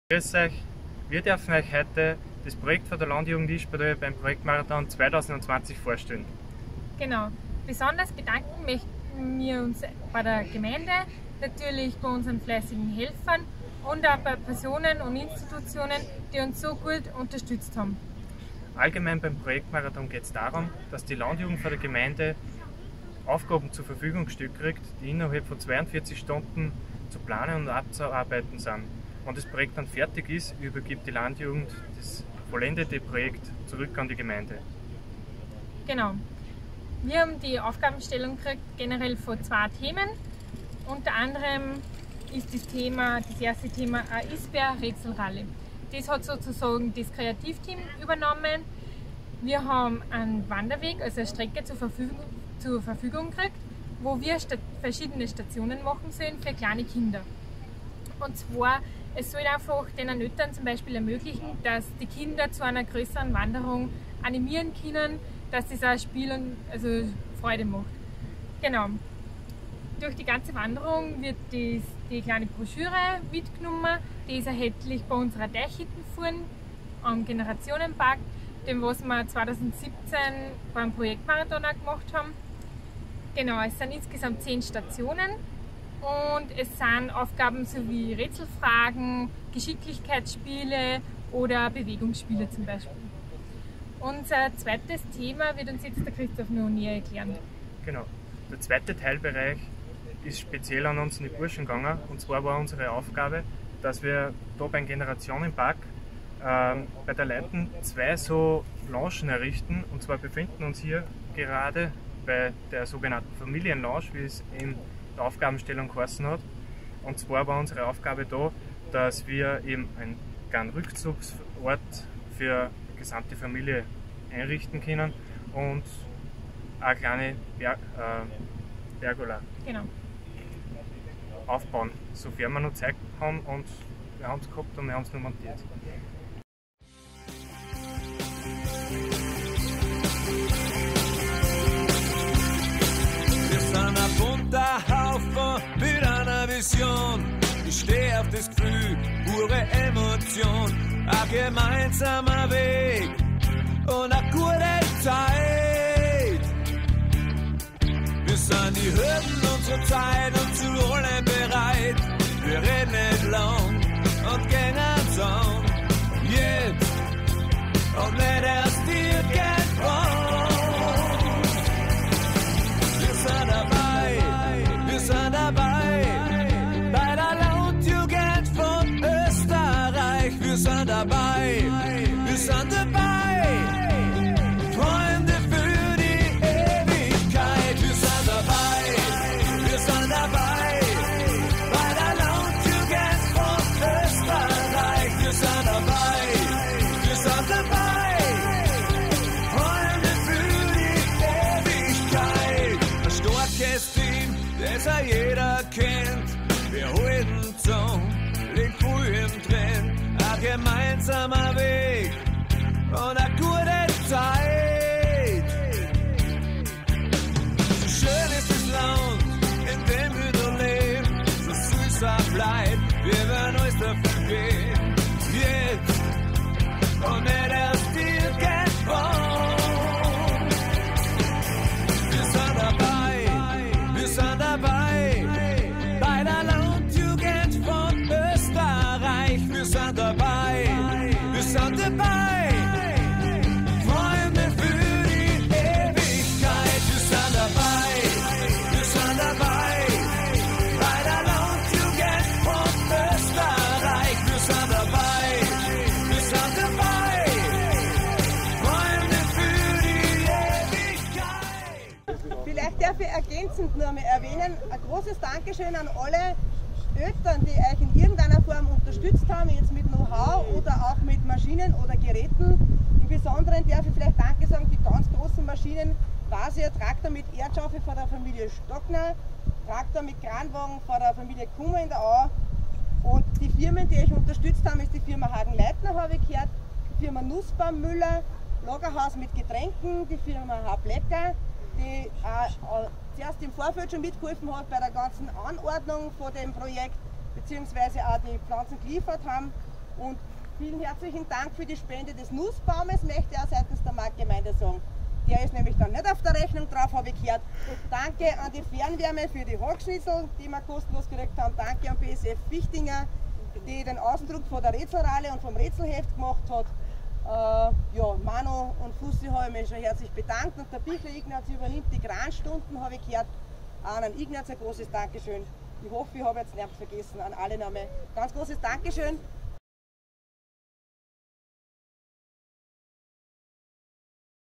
Projektmarathon 2020 - Landjugend Yspertal / Aftermovie
Endlich ist es soweit!
Wir dürfen euch stolz unseren "Aftermovie" zu unserem Projekt zeigen.
Vom 11. - 13. September arbeiteten wir an unserem Projekt "Ein perfekter Nachmittag: Eine Rätselrunde mit dem Ysbär und anschließender Erholung in der Familien- und YsBär-Lounge."

Noch mehr Infos zu unserem Projekt gibts auf unserem Blog und Sozialen Medien: